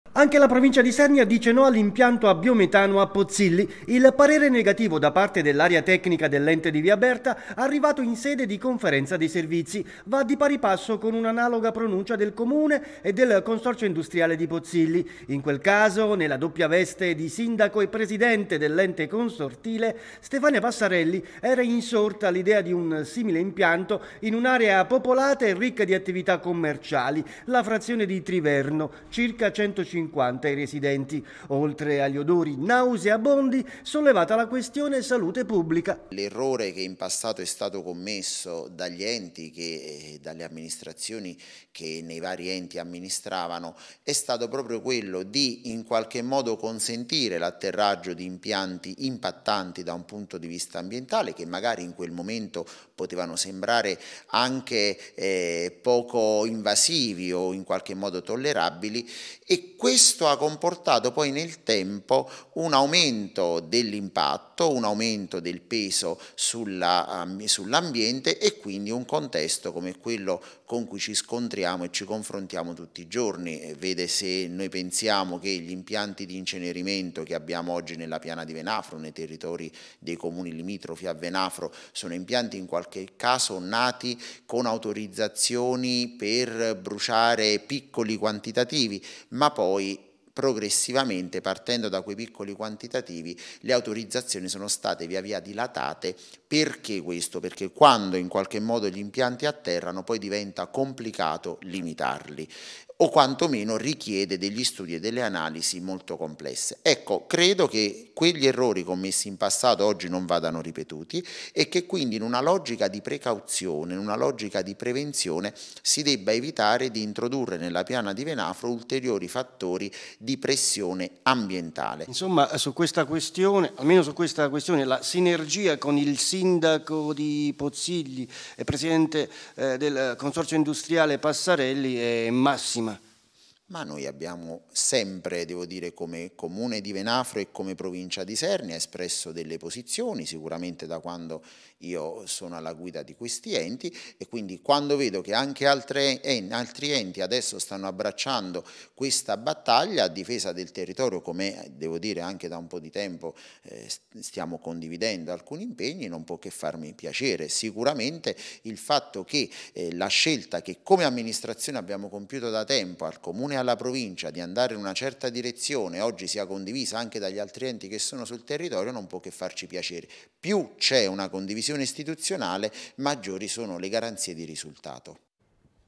La Provincia stoppa il progetto della centrale a biometano a Pozzilli, Ricci: "La precauzione..."
Dopo il Comune di Pozzilli e il Consorzio industriale,  anche la Provincia di Isernia da parere negativo al progetto di un impianto a biometano in territorio di Pozzilli. Ricci: “Ha prevalso la precauzione!”